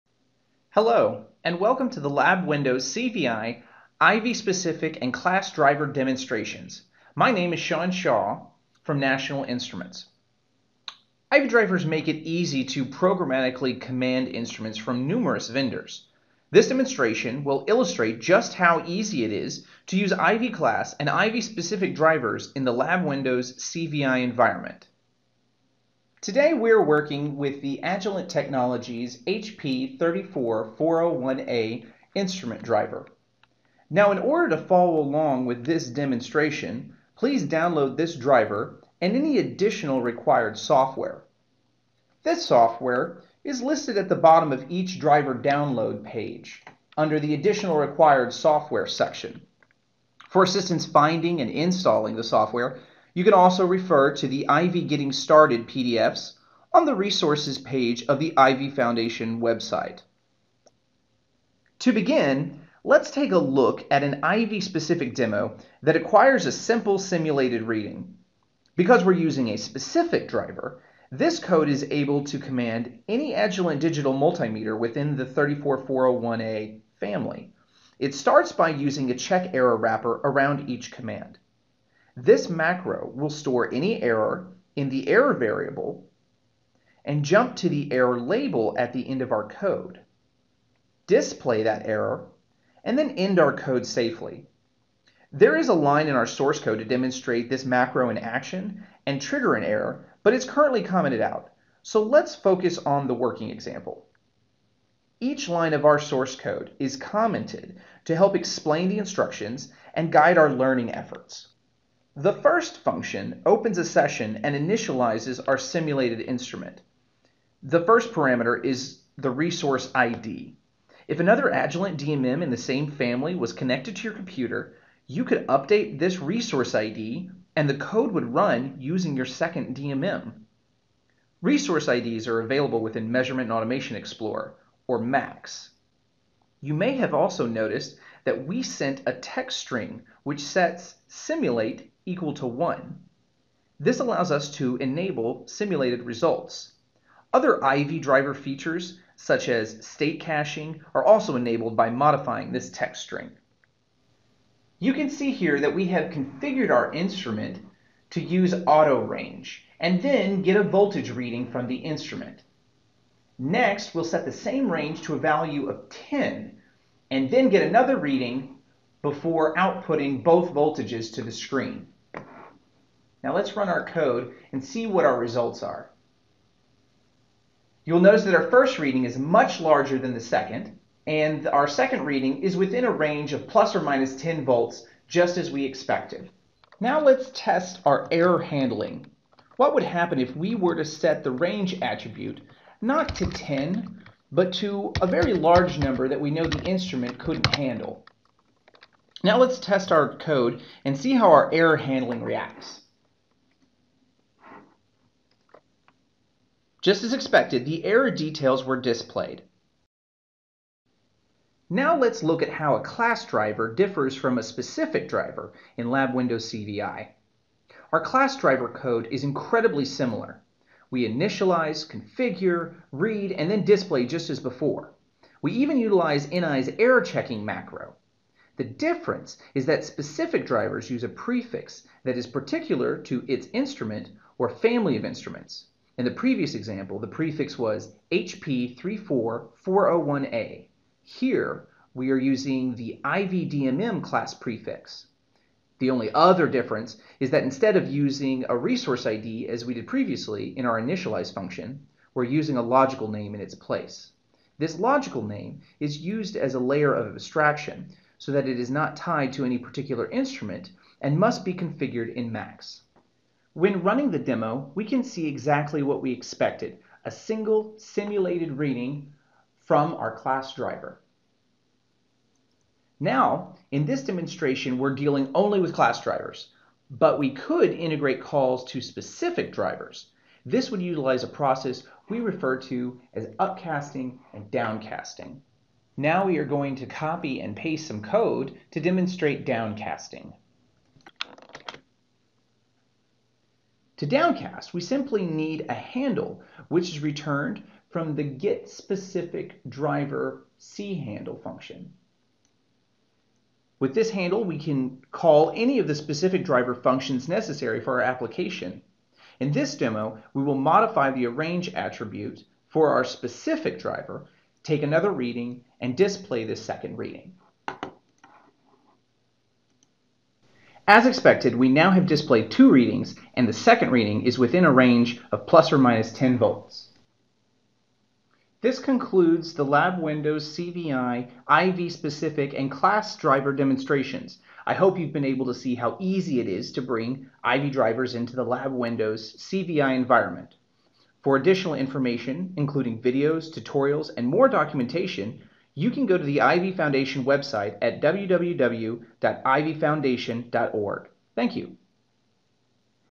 How to Use IVI Specific and Class Drivers in LabWindows CVI
This demo shows how to integrate IVI specific and IVI class drivers.